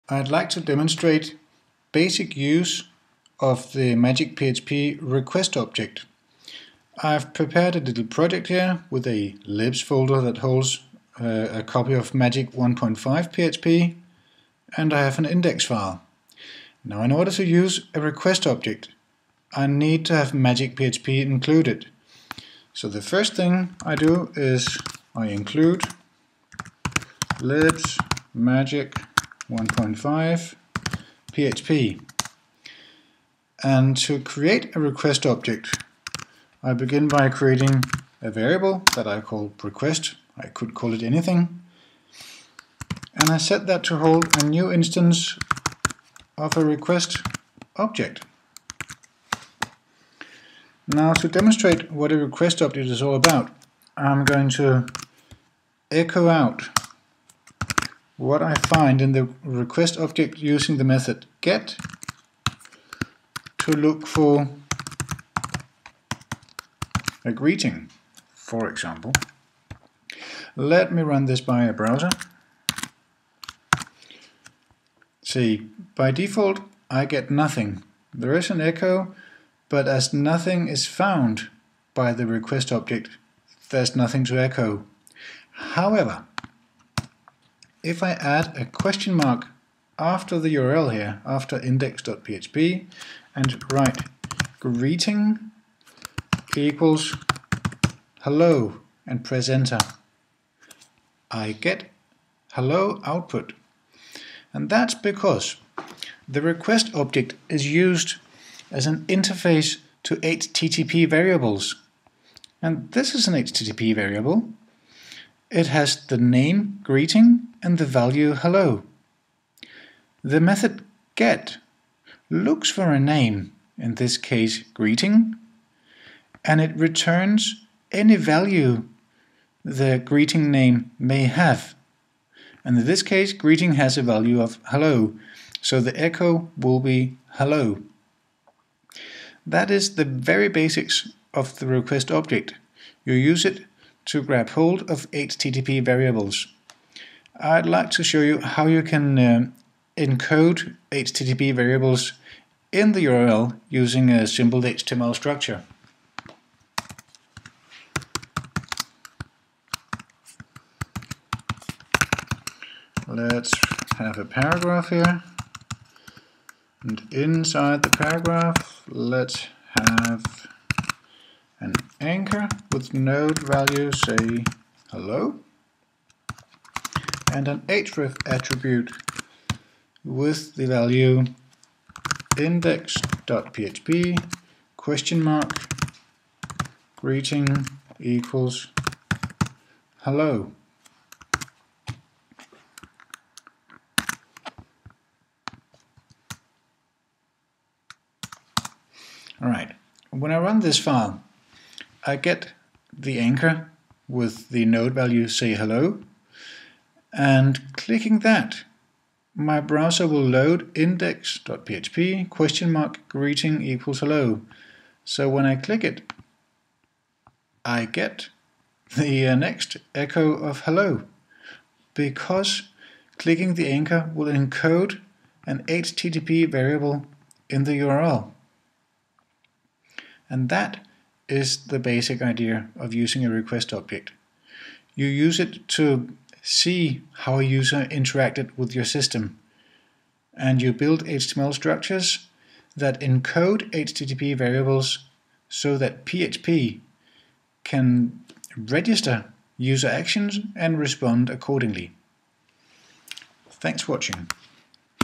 introducing the request object
Basic introduction to using the Request object from the magic PHP framework.